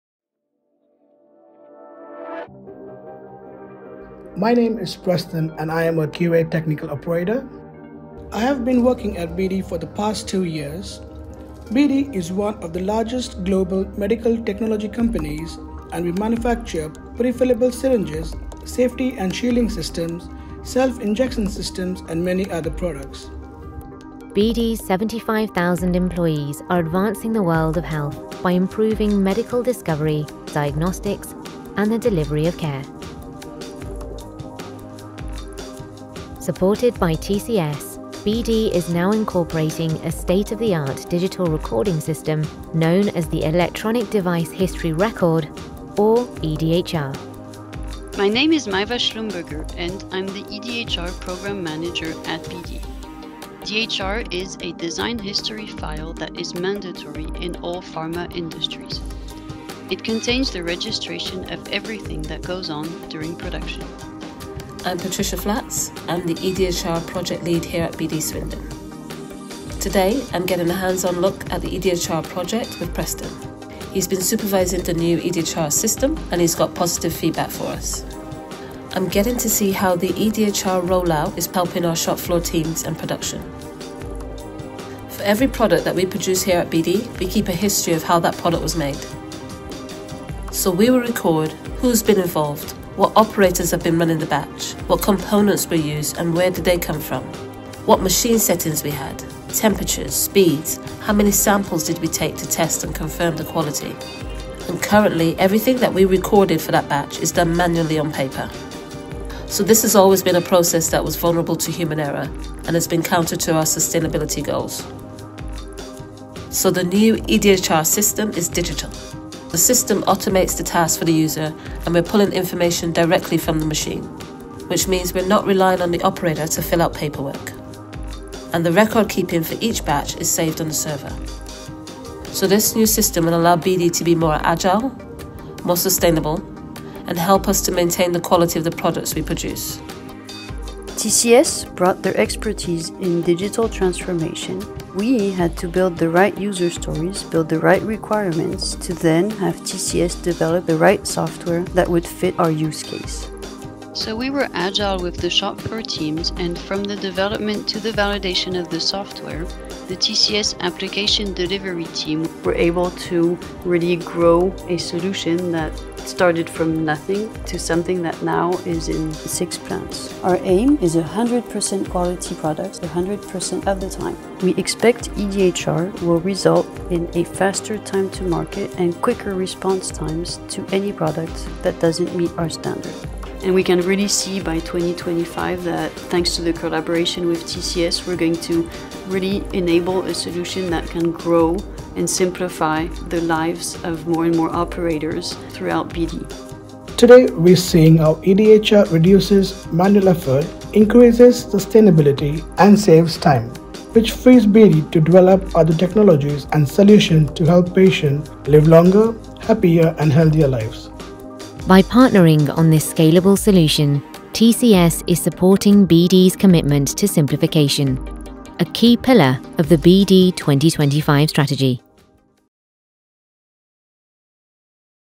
TCS Digitizes BD Medical's Pharmaceutical System Device History Records
BD Medical is a global medical technology company. Here’s how we helped BD Medical – Pharmaceutical Systems move from manual, paper recording to a sophisticated electronic device history record system that boosts sustainability and increases productivity.